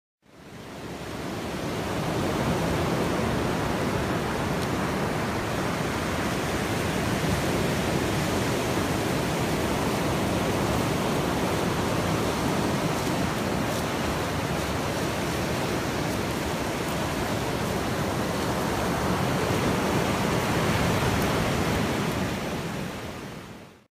Beached shark - Lido Beach 6/25/15
Came across a beached sand shark during an early morning walk at Lido Beach, Long Island. Got him safely returned to the water.